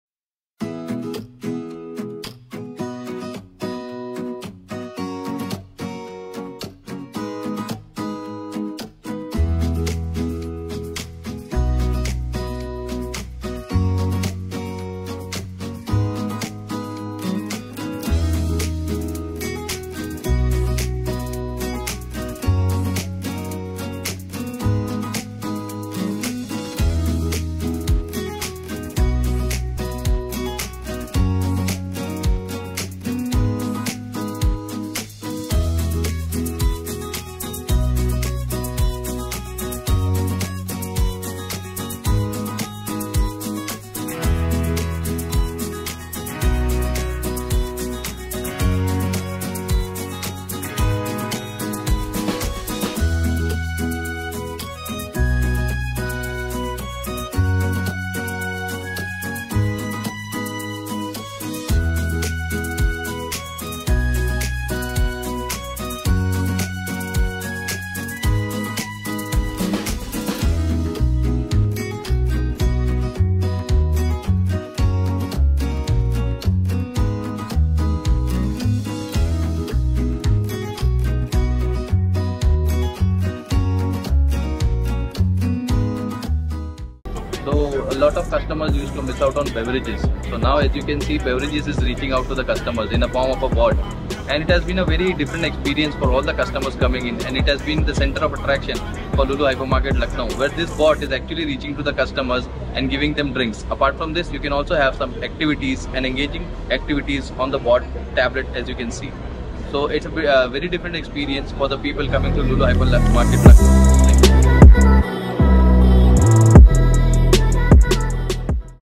Artiligent CokeBot | Custom Crafted Robot | Coca-Cola | Lulu Hypermarket
Artiligent takes pride in crafting an exclusively designed custom Robot for the @Coca-Cola  deployed at @LuluGroup  Hypermarket, Lucknow, India.

Another feather in the hat on the award winning @TemiRobot   Platform.